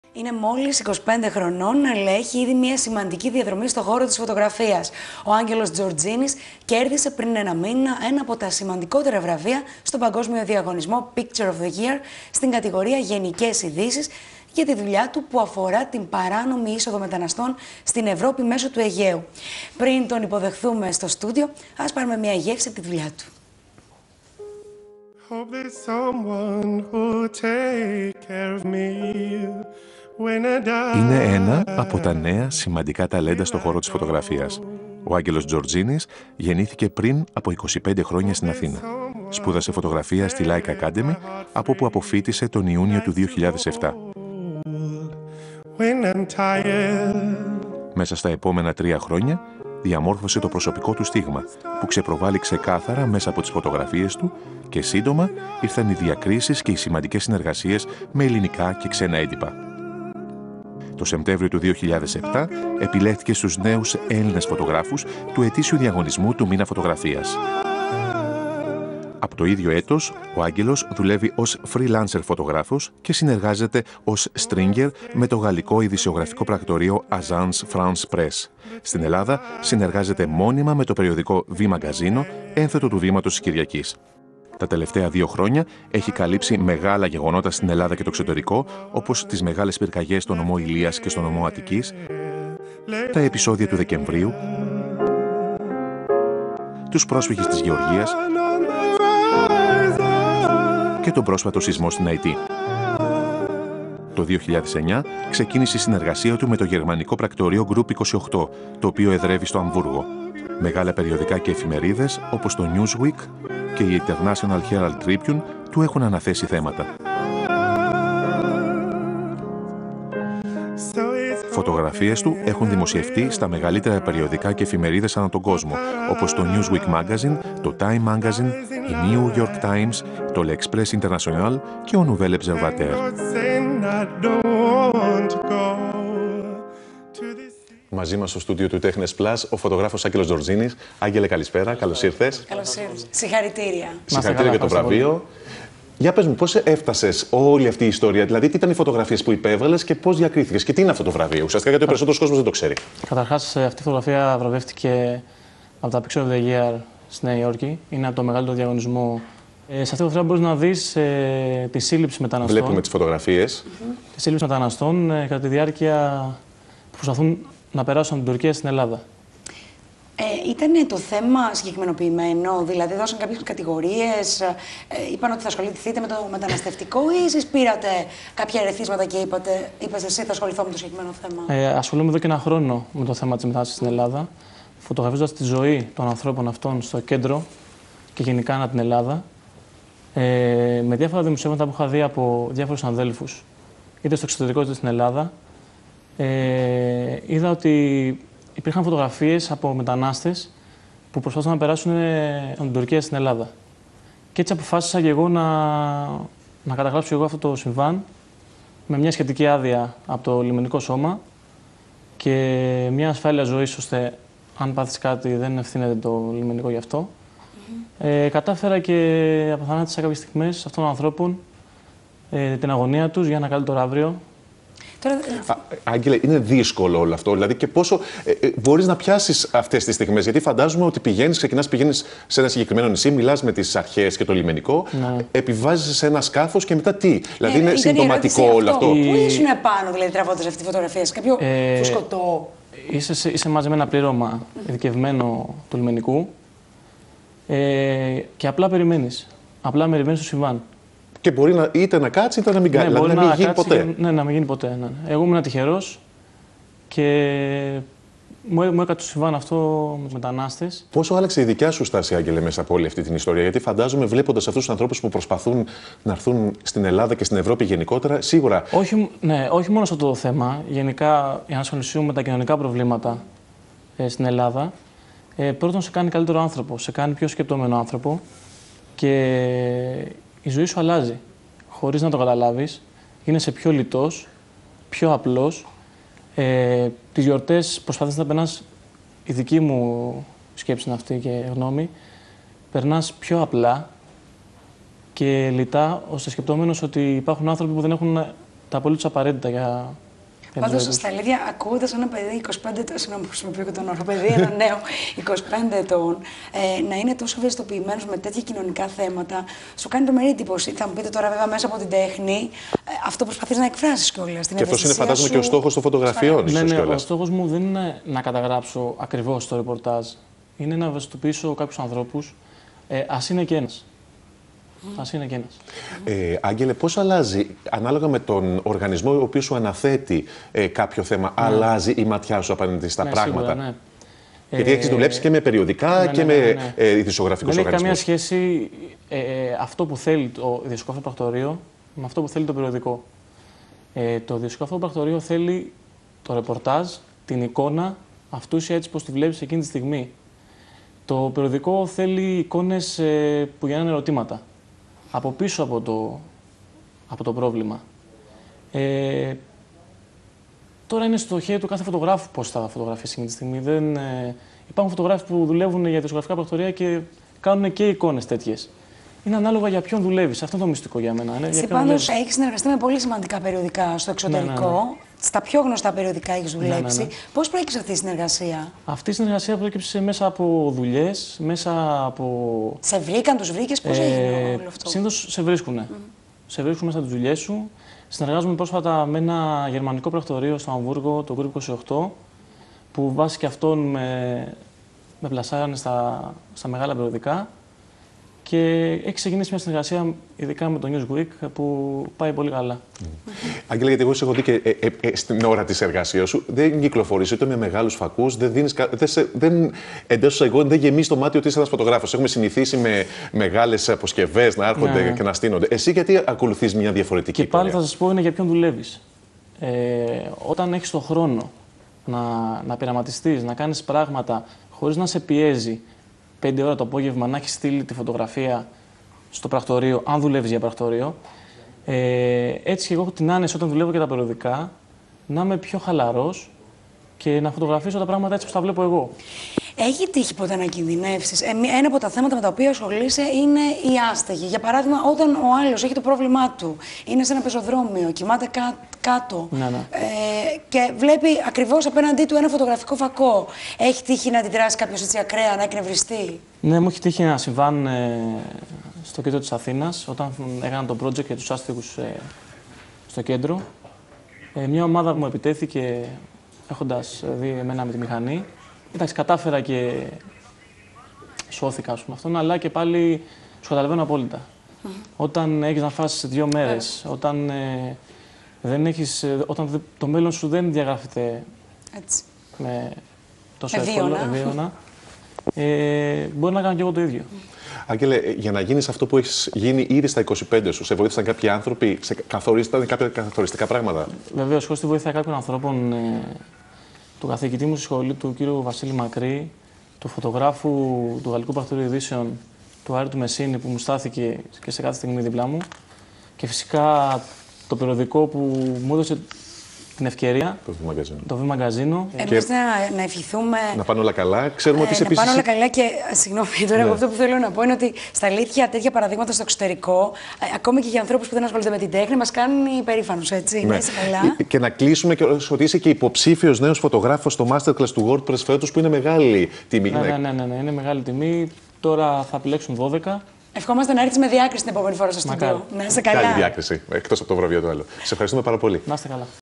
Angelos Tzortzinis interview EΡT .mov
Angelos Tzortzinis talks about the award, which he won in the competition POYi pictures of the year international 2010. The awarded photo is about illegal immigrants in the Aegean sea.